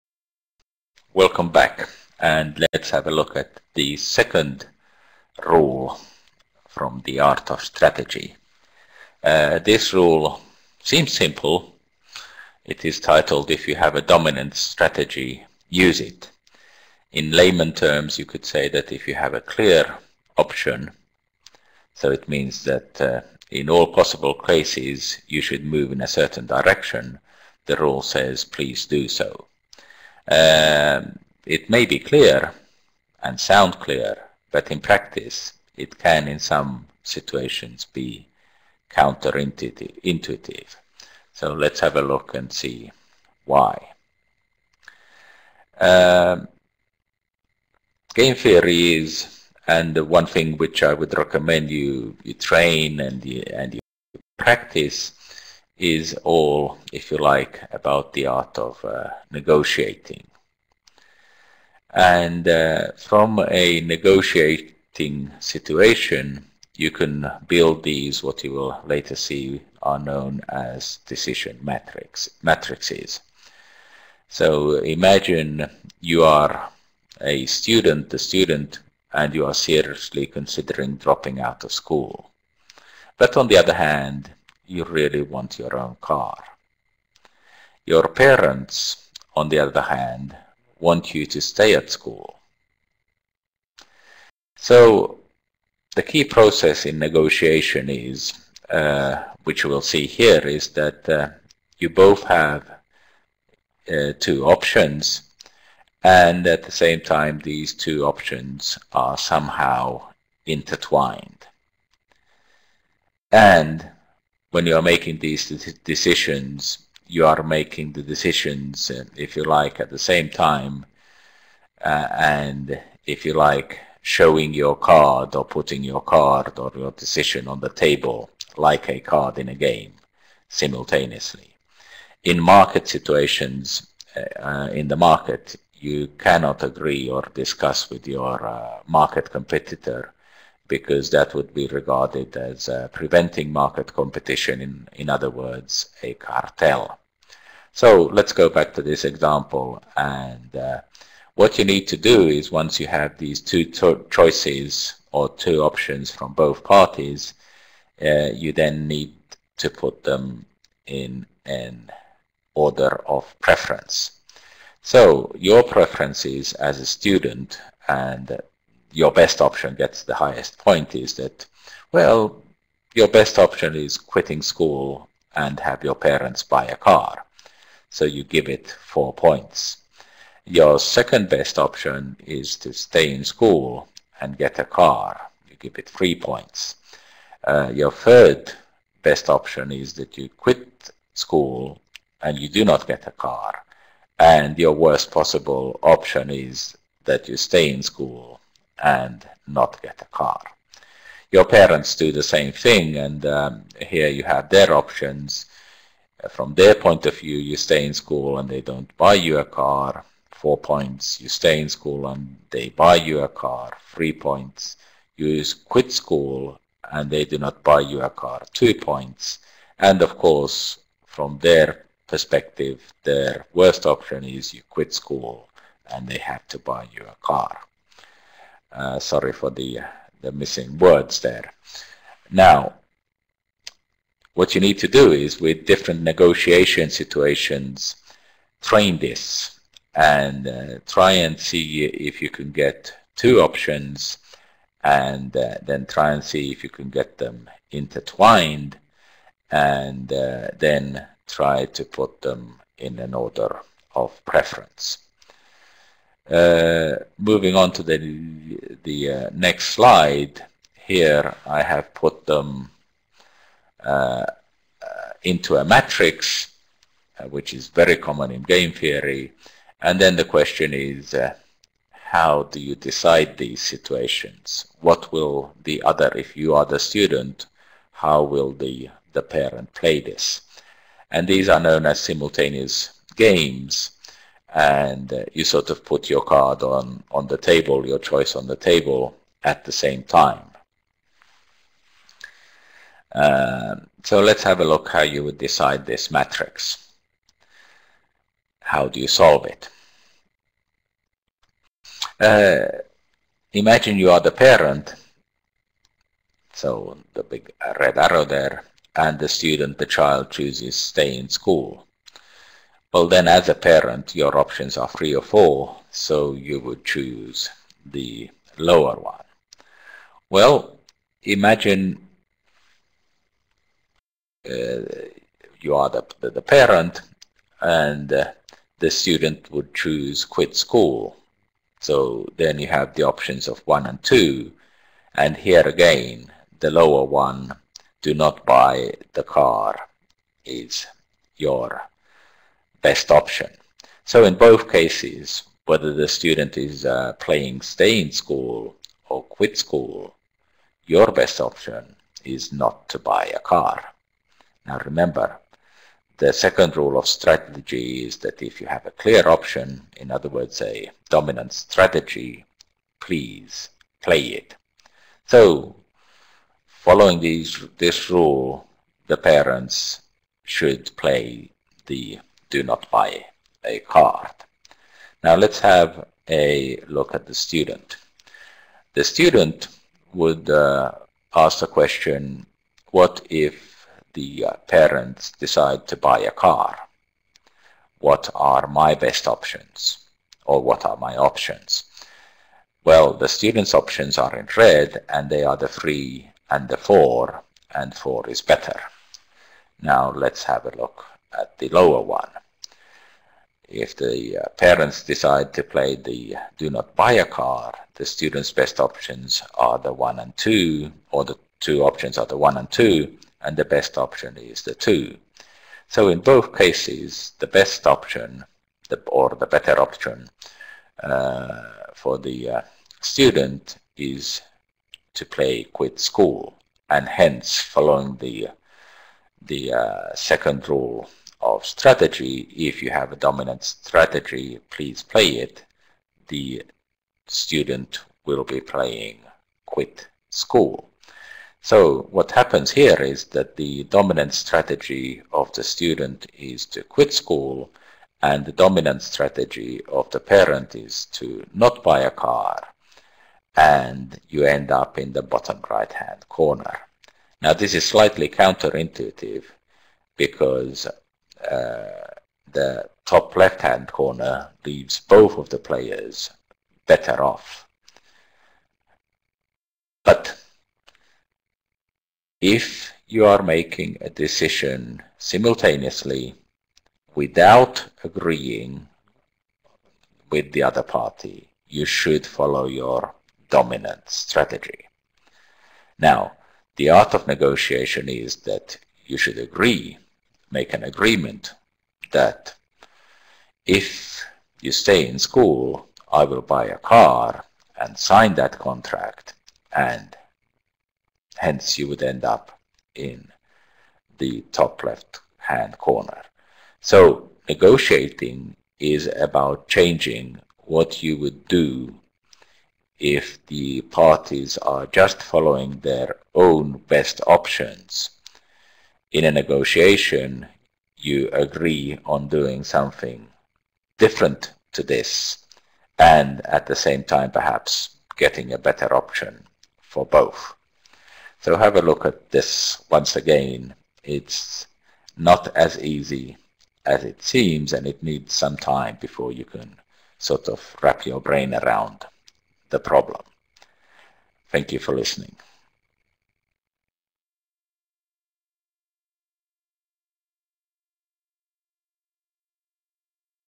Laurea Saarikoski Ville 36 rule 2 dominant strategy
From the book the Art of Strategy Rule 2 part A